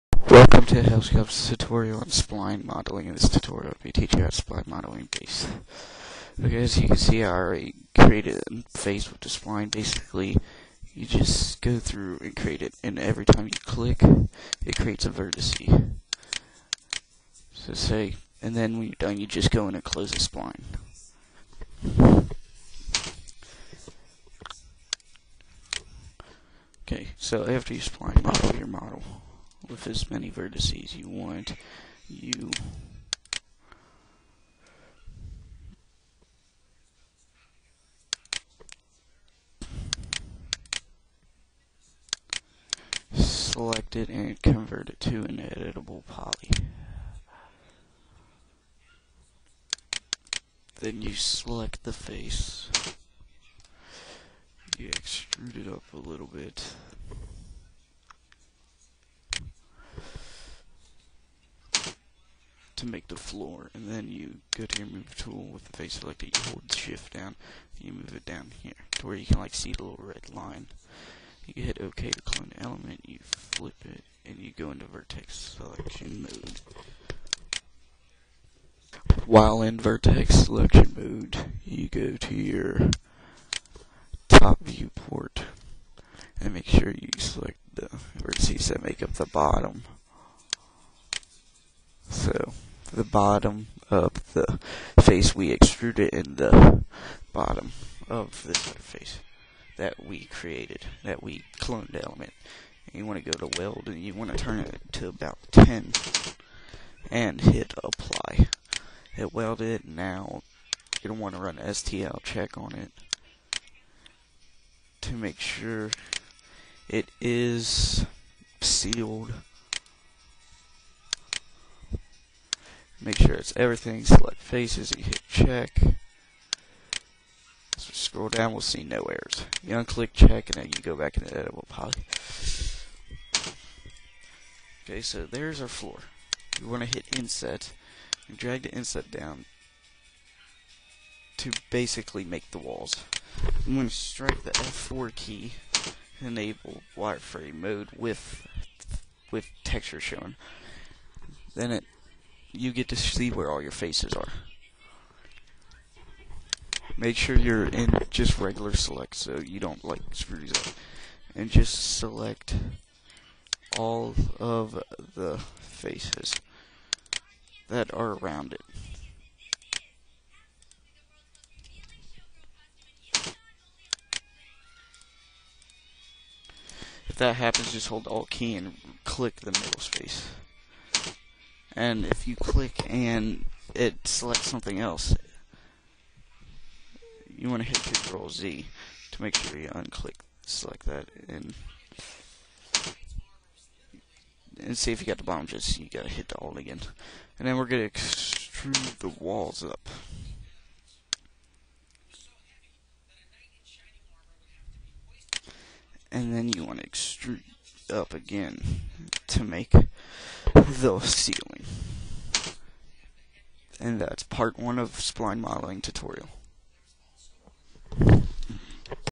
Spline Modeling - Part 1
This is Hëlls§¢øÞë's spline modeling tutorial. This is part 1 of the 3-part tutorial. The next part of the tutorial can be found here: